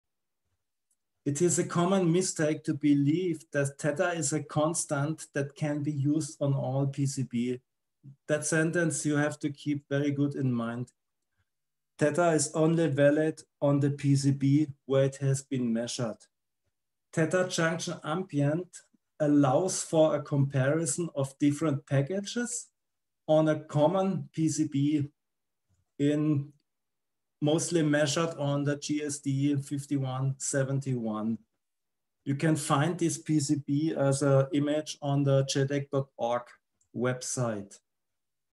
Tips for Theta Values on PCBs
Automotive systems of the future will demand higher power and integrate more electronics, making thermal management a big area of concern. Unfortunately, it can be quite confusing trying to understand how a datasheet’s thermal parameters translate to your real design – when should you use theta instead of psi, for example, and how to convert PCB temperatures to what’s going on inside the IC.